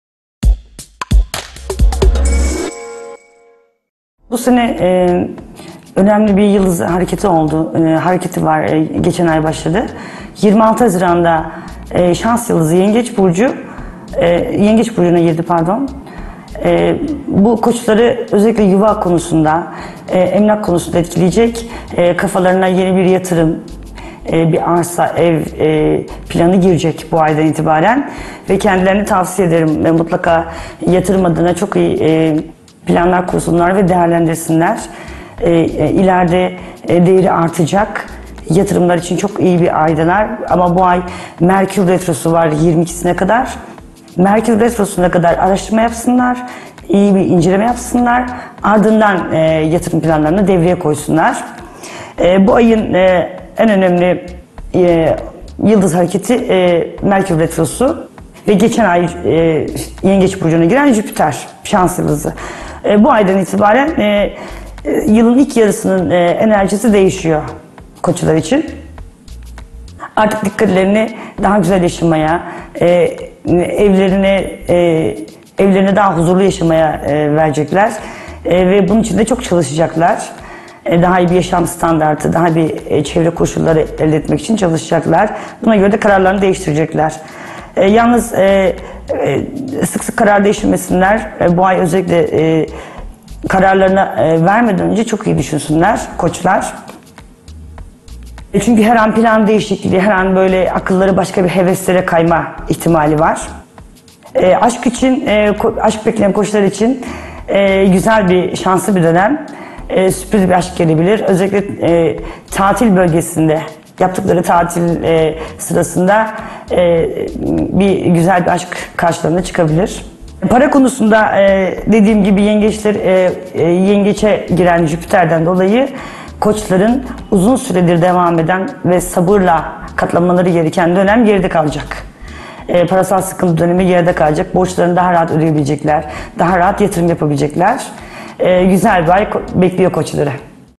Koç burcunu 2013'ün Temmuz ayında neler bekliyor?
Koç burcu mensupları Temmuz ayında yatırım planları yapabilir.